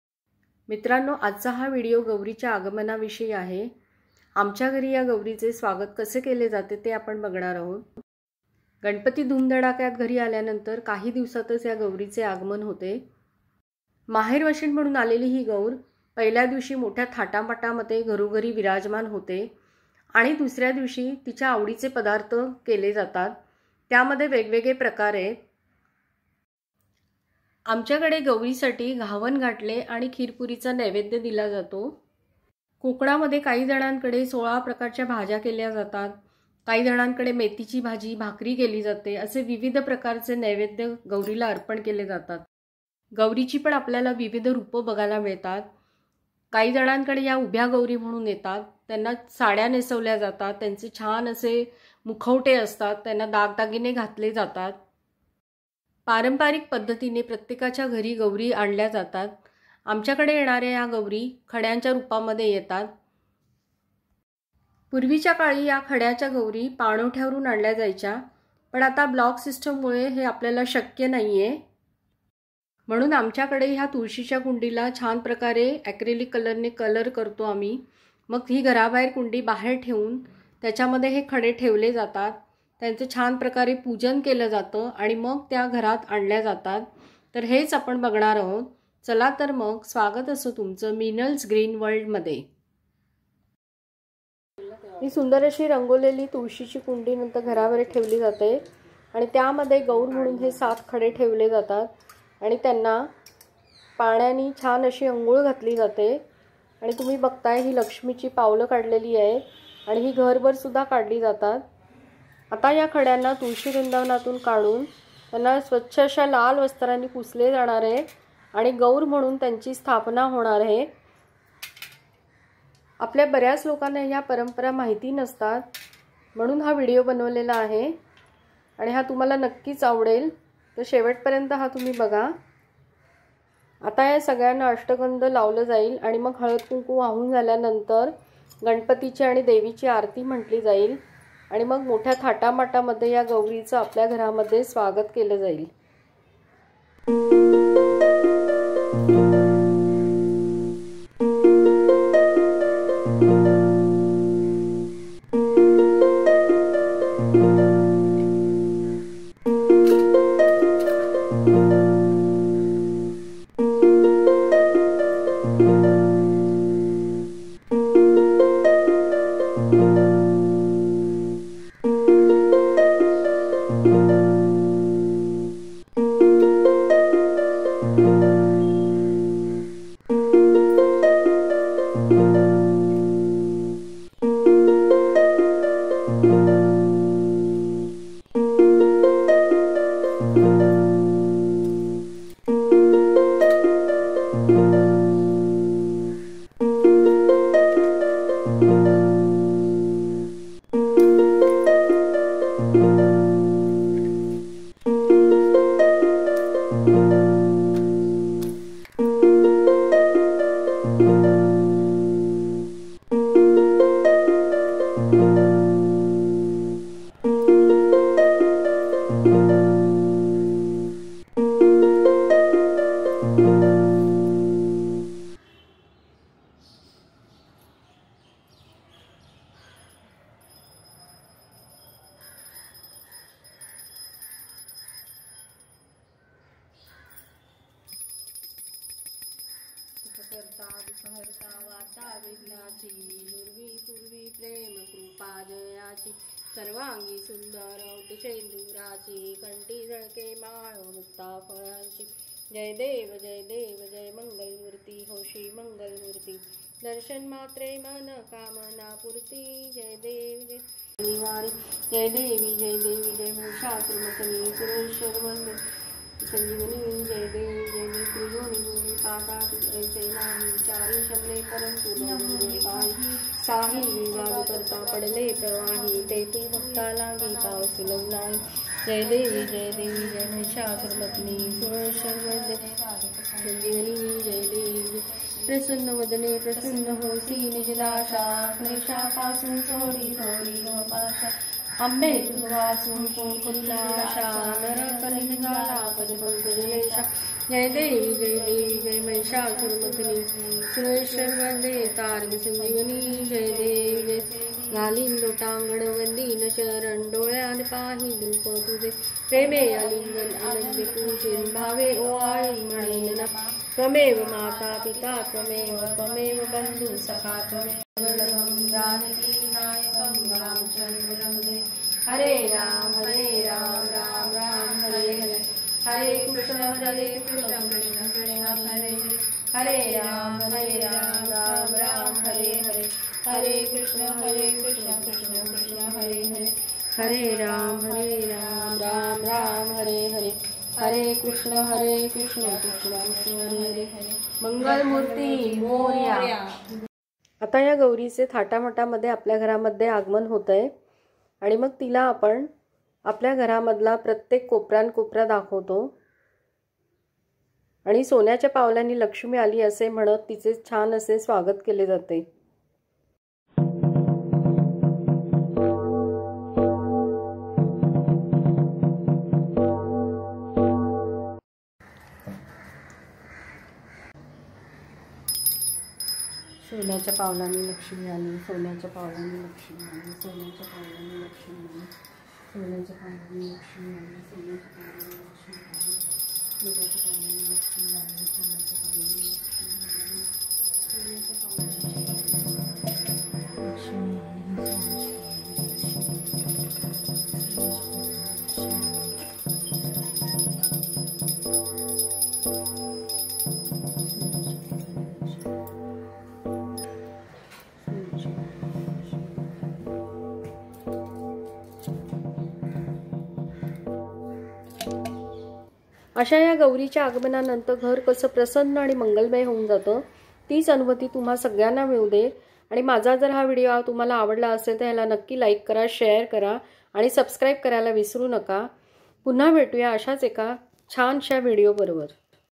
Gaurinche Aagman from Tulasi plant ,खड्यांच्या गौरींचे तुलसि वृनदावन मधून आगमन,Meenal's Green World
Gaurinche Aagman from Tulasi plant ,खड्यांचया गौरींचे  तुलसि वृनदावन मधून आगमन,
Meenal's Green World.

How we welcome our Gauri ,
Information about  Gauri,
Preparation  for Gauri Aagman,
Preparation  of Tulasi pot to bring  Gauri ,
Ganpati and Devi Aarti ,